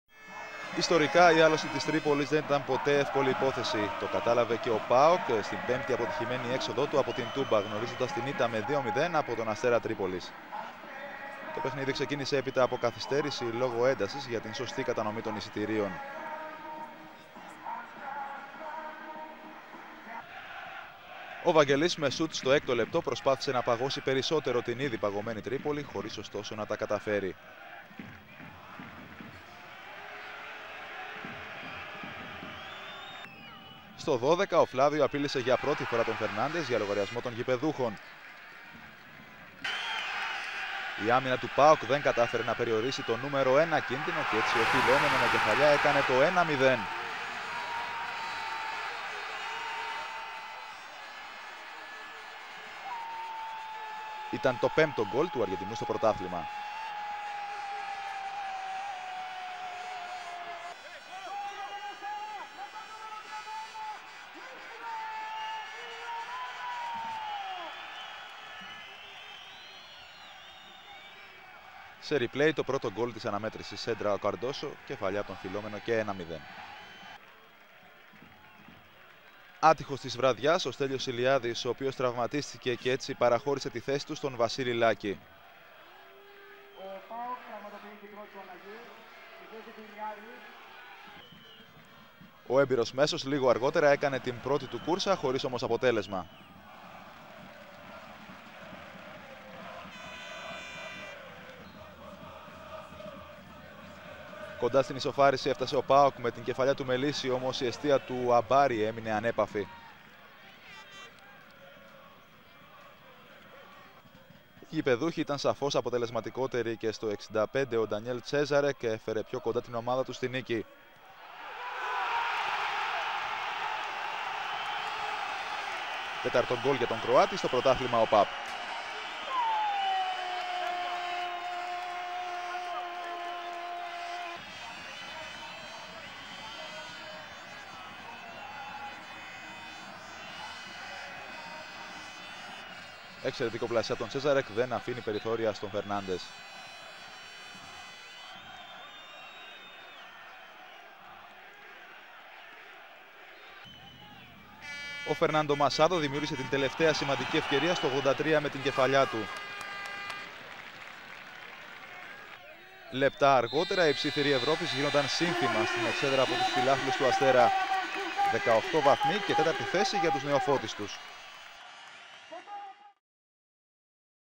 Αστέρας Τρίπολης - ΠΑΟΚ(2-0), 02/12/2007
Δείτε τα καλύτερα στιγμιότυπα από τον αγώνα Αστέρας Τρίπολης - ΠΑΟΚ της σεζόν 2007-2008